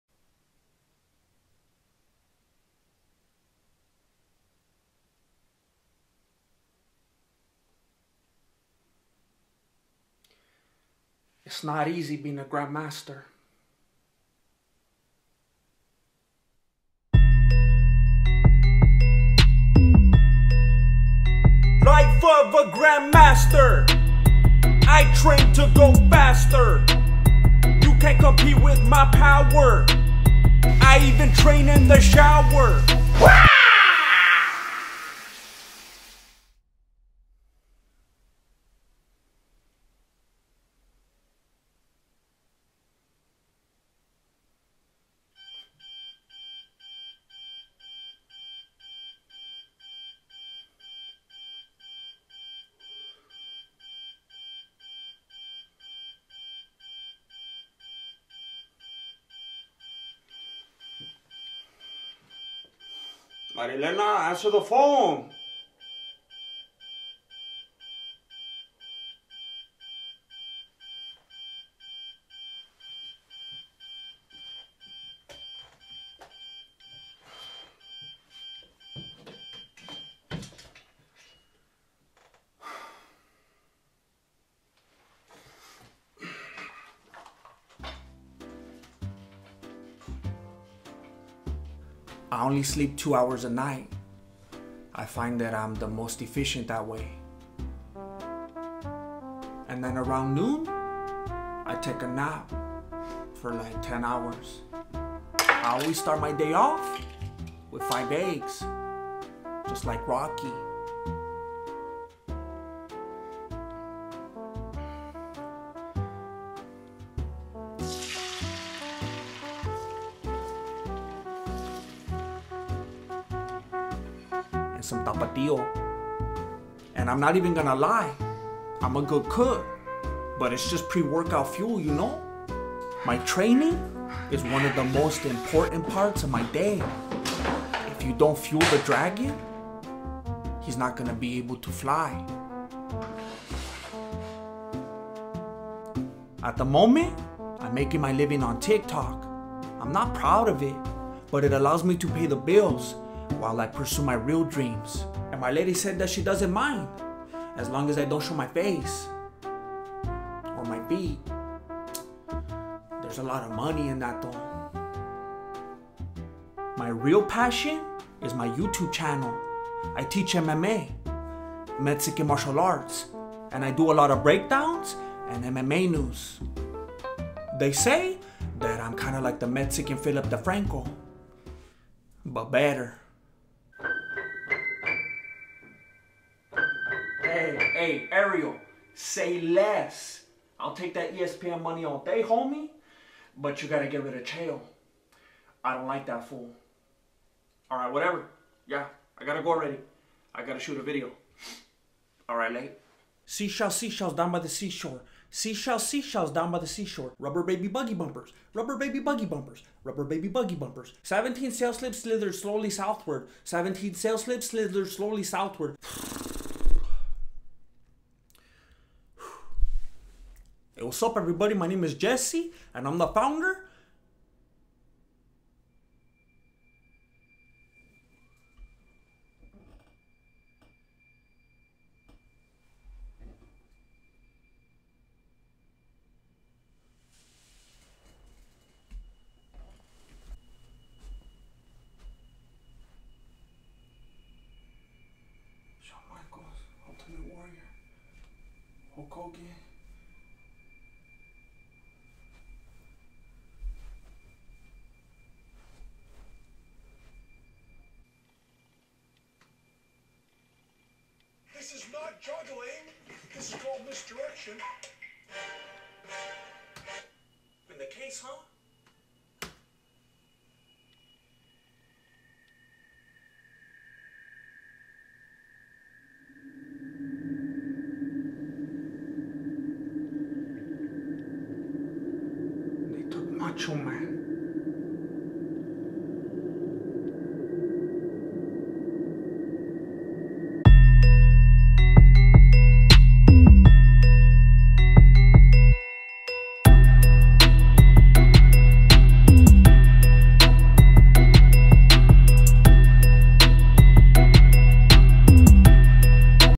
Life of a Grand Master: Episode 1
Life of a Grand Master is a peek into the life of the founder and Grand Master of the Mexican Martial Arts.